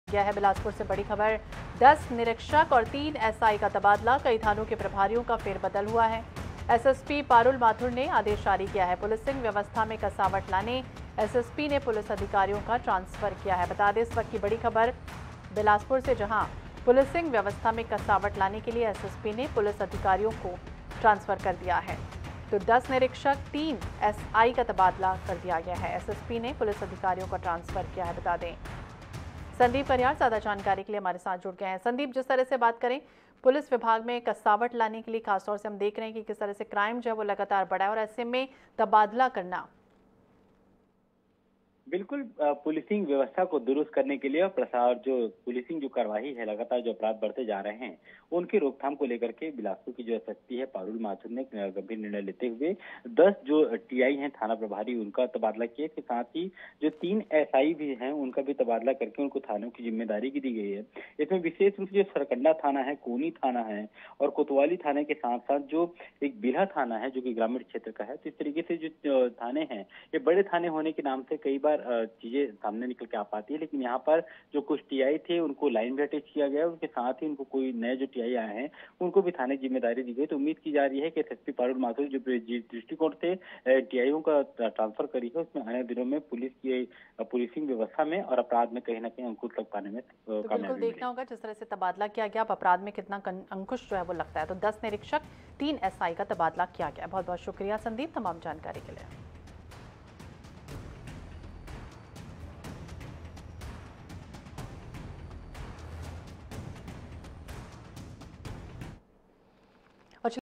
Chhattisgarh Police News || SSP Parul Mathur ने जारी किया आदेश, कई थानों के प्रभारियों का हुआ फेरबदल
SSP Parul Mathur ने जारी किया आदेश, कई थानों के प्रभारियों का हुआ फेरबदल

Chhattisgarh Police News,
Chhattisgarh Police Department,
CMO Chhattisgarh,
Chhattisgarh Latest News,
Latest News,